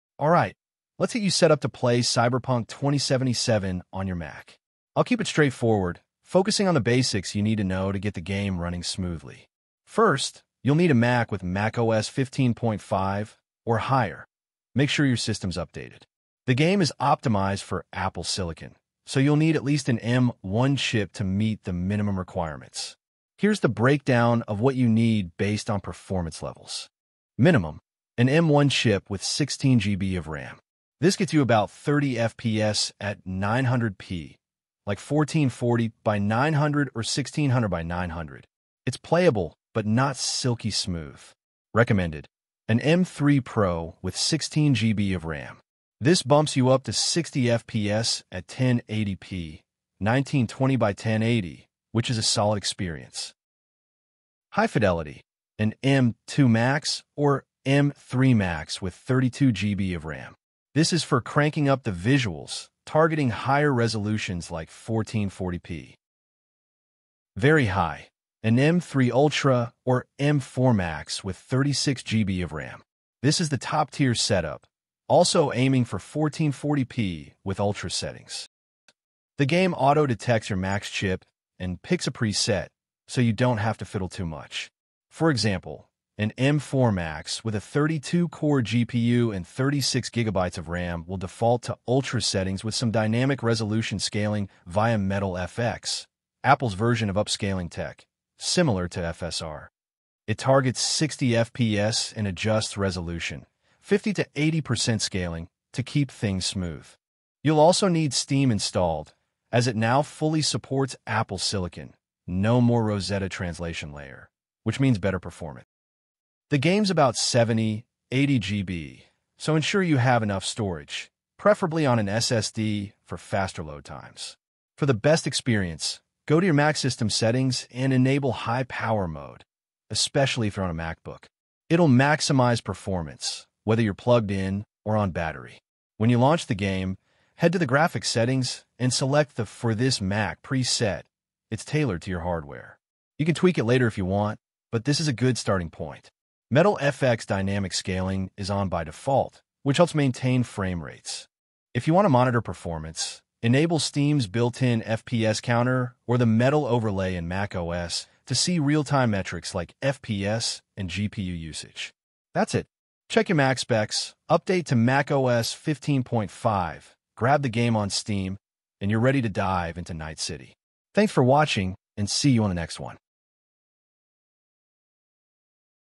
How to Install Cyberpunk 2077 on macOS
In this video we will tackle on How to Install Cyberpunk 2077 on macOS along side with its preferred requirements to run this game.

Our Wishlist Setup:
- Laptop: MacBook Pro M4
- Desktop: Mac Mini M4
- Monitor: Apple Studio Display
- Apple Magic Keyboard
- Apple Magic Mouse